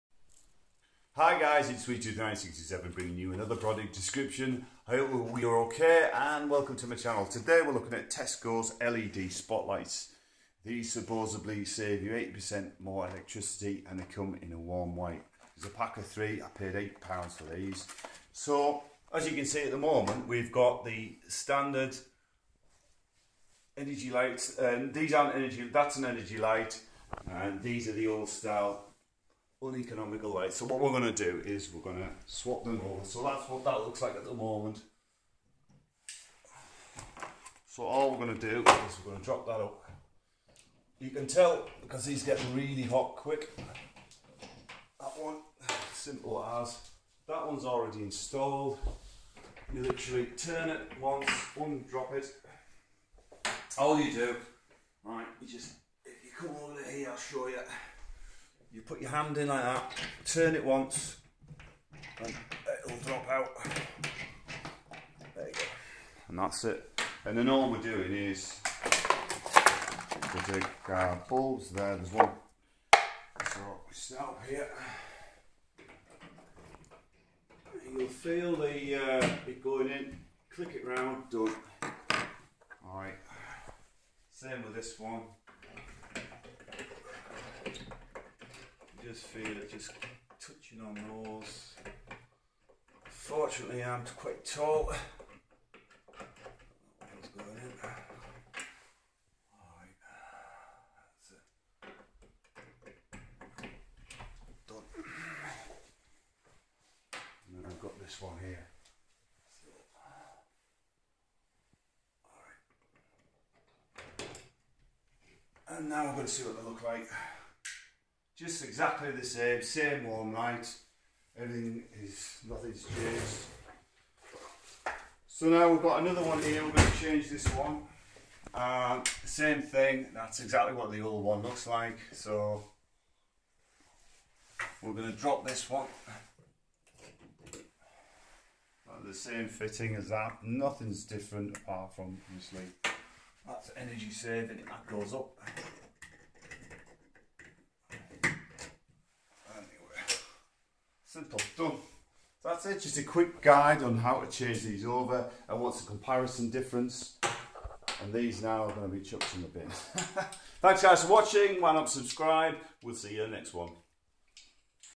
save money with Tesco warm white led spot lights g10 230 3.6w =35w easy fitting demo
this is the easy way too replace those expensive hot leds around the home with this 80% more efficient t bulbs from tescos